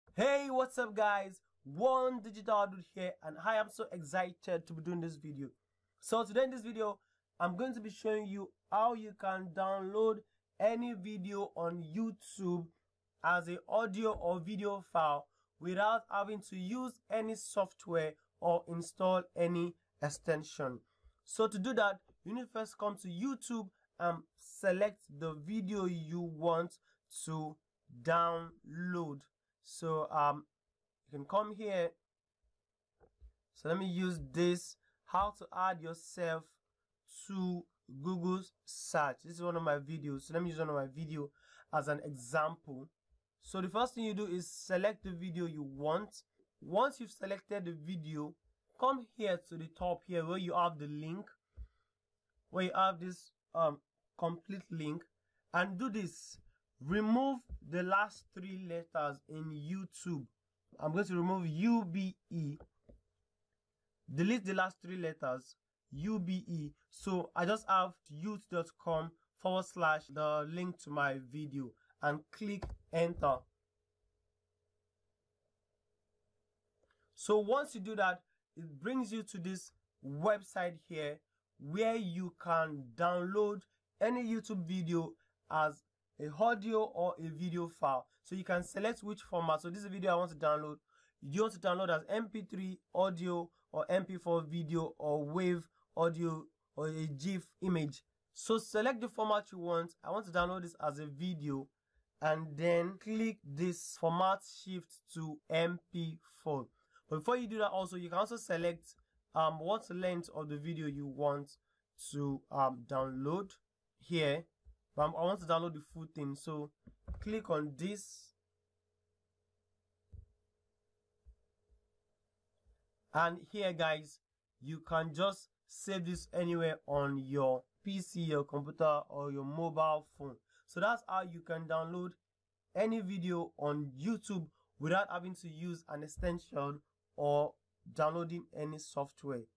How to Download YouTube Video in Just 1 Click
In this video, I explained how you can download YouTube videos without having to install an extension or a software.